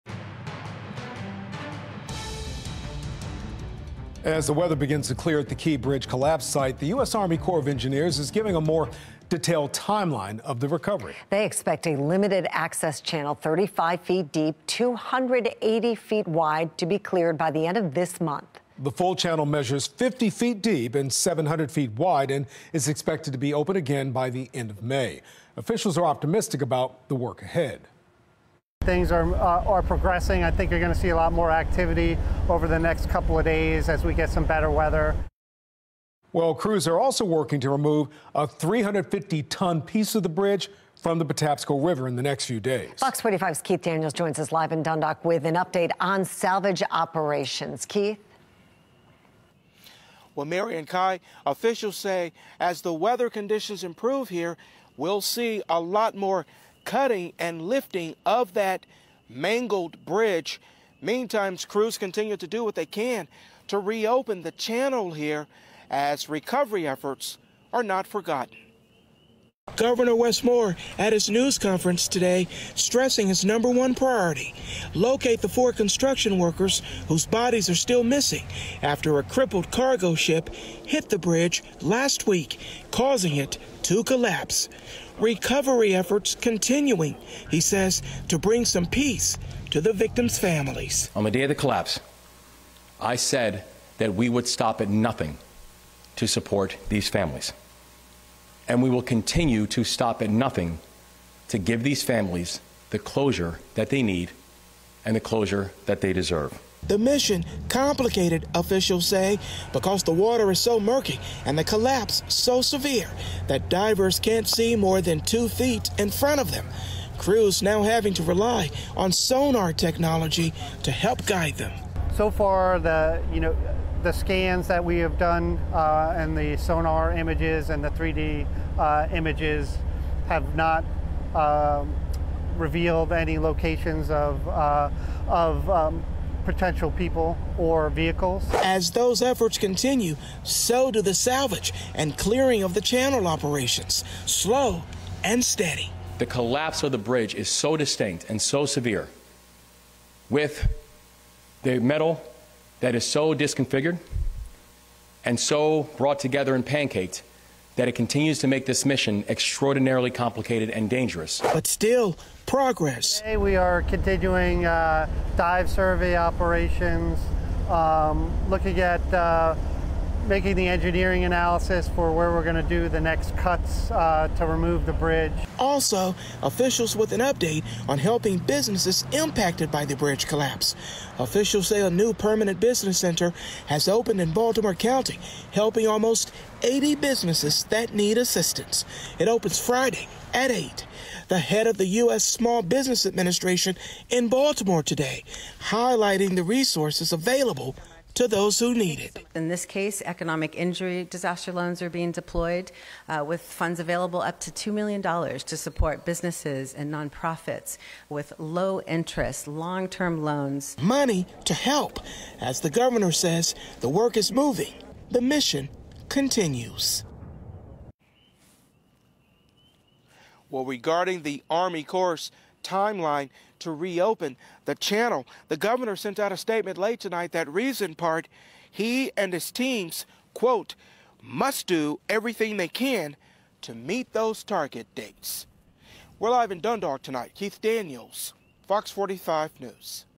Tentative timeline set to reopen Fort McHenry Channel after Key Bridge Collapse
There's now a tentative timeline as to when the Fort McHenry Channel could reopen following the Key Bridge collapse. The U.S. Army Corps of Engineers released the news Thursday night.

According to USACE officials, they expect to open a limited access channel 280 feet wide and 35 feet deep, to the Port of Baltimore within the next four weeks--by the end of April. This channel would support one-way traffic in and out of the port for barge container service and some roll on/roll off vessels that move automobiles and farm equipment to and from the port.